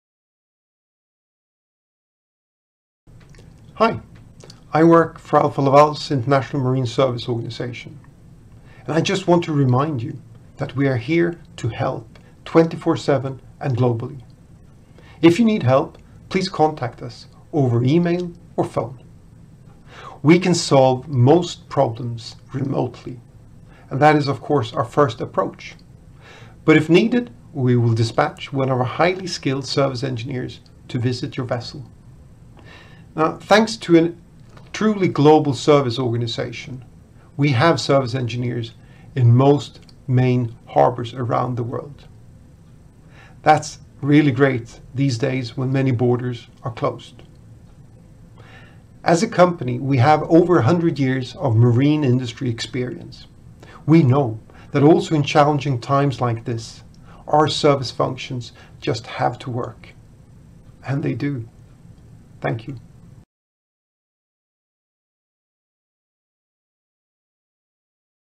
Marine service in challenging times
How do you bring service to vessels when access is limited? Ask Magnus Nordin, our International Marine Service Manager. “We know that, also in challenging times like this, our service functions just have to work,” he says.